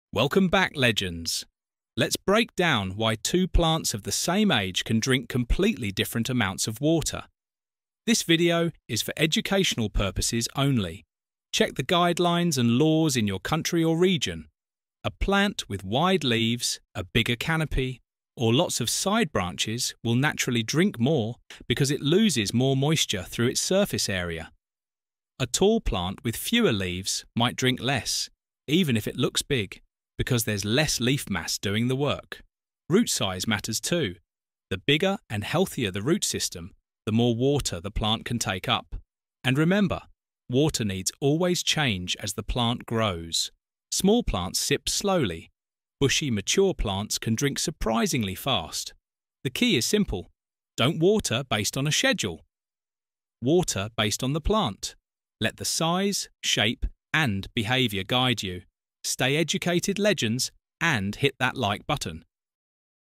You’re Feeding Your Cannabis WRONG — Here’s the Truth
Welcome back legends! In today’s episode we break down one of the most confusing parts of growing any plant — understanding how feeding, watering, pH, EC and PPM actually work. This video is designed to help beginners understand the science, avoid guesswork, and recognise what their plants may be trying to tell them.

We’re keeping everything educational only, focusing on how plant nutrition works, why water quality matters, and how different growth stages naturally affect nutrient needs. No tips, no instructions — just clear explanations to help you learn safely.

🌱 What we cover in this video:
 • What EC, PPM and pH really mean
 • How water quality affects overall plant health
 • Why seedlings, veg plants and flowering plants all behave differently
 • Common feeding misunderstandings beginners often have
 • The importance of understanding your nutrient levels before you feed
 • Why an EC/PPM meter can help reduce guesswork for learners  ￼

🌿 Why this matters:
Learning how plants take in water and nutrients is one of the biggest steps toward understanding plant biology. This video simplifies the process so beginners can build confidence without doing anything risky or unsafe.

This content is strictly for education and harm-reduction. Nothing in this video shows, promotes, or instructs how to grow, cultivate, or produce anything illegal in the UK. Always follow your local laws.